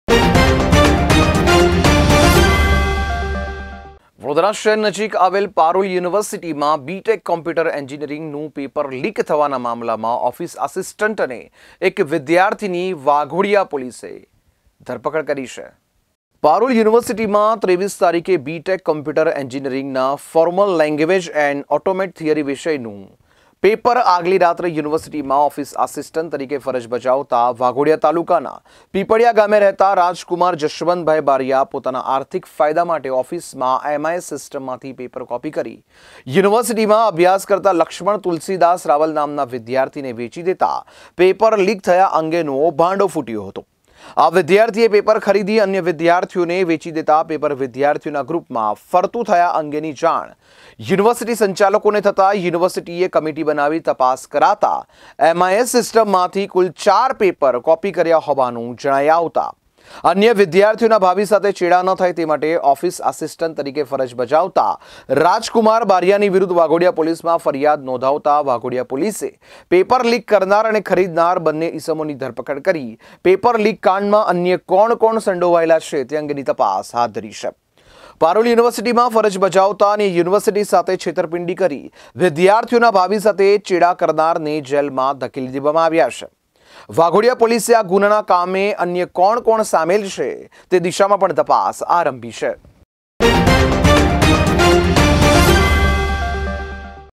પારુલ યુનિવર્સિટીમાં પેપર લીક કાંડમાં બે ની ધરપકડ કરતી વાઘોડિયા પોલીસ | G One News
વડોદરા શહેર નજીક આવેલ પારુલ યુનિવર્સિટીમાં બીટેક કોમ્પ્યુટર એન્જિનિયરિંગ નું પેપર લીક થવાના મામલામાં ઓફિસ આસિસ્ટન્ટ અને એક વિદ્યાર્થીની વાઘોડિયા પોલીસે ધરપકડ કરી છે